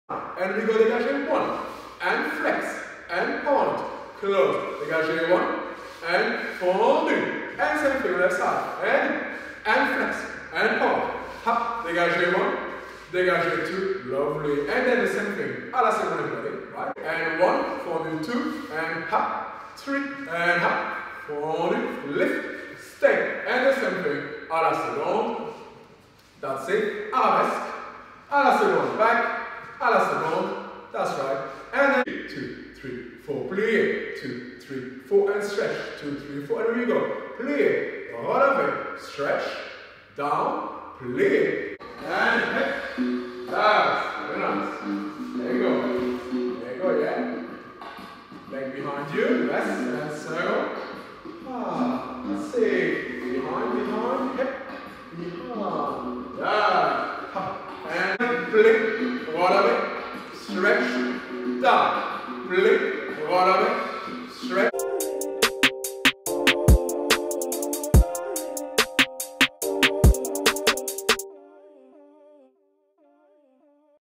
Horton Technique with MIU: Foot Work Combination
Are you looking for Horton resources or an online Horton Technique Certification Programme? Join MIU!

➡️Horton Technique with MIU🎓
💙Foot Work Combination with our talented dancers in action!

➡️ Make your Horton warm-up unique by combining different exercises in a short combination of strengthening movements targeting the feet and ankles.

💙 Learn all about this class in our online courses at M-Intensive University!
Ready to become a member?